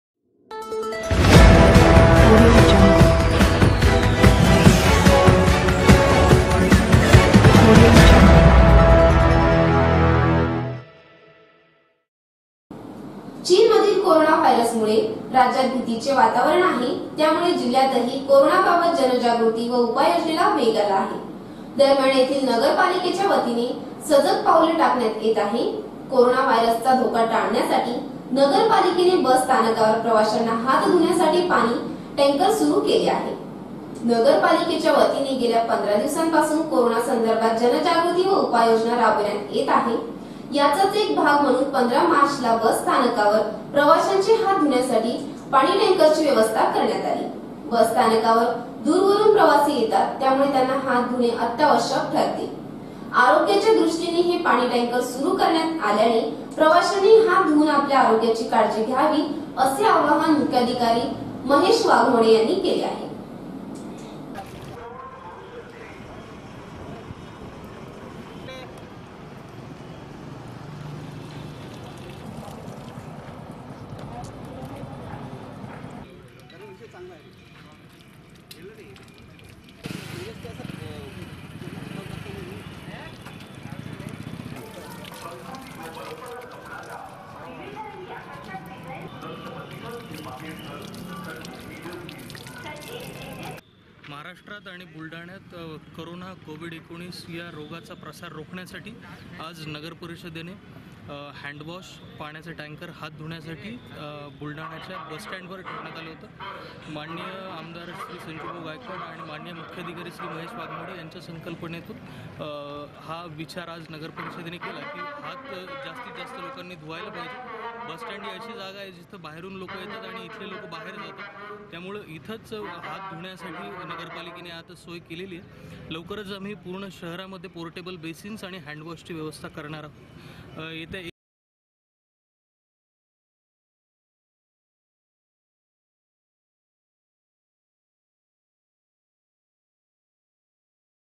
बुलढाणा - कोरोना : हात धुण्यासाठी न पा ने बस स्टॅंडवर ठेवले पाण्याचे टॅंकर
बुलढाणा  -  कोरोना :  धुण्यासाठी न पा ने बस स्टॅंडवर ठेवले पाण्याचे टॅंकर